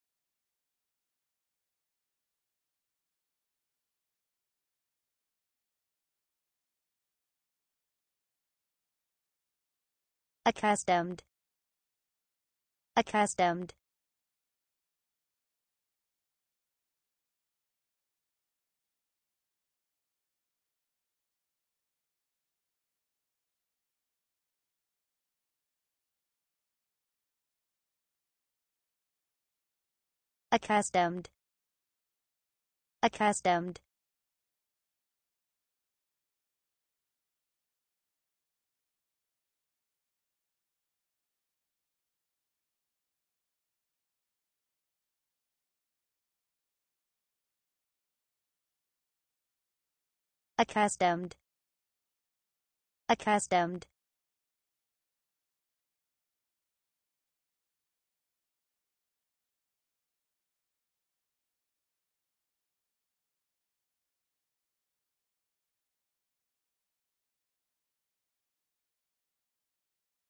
How to say Accustomed in English? - Pronunciation Owl
How to say Accustomed in English? Learn the pronunciation of Accustomed!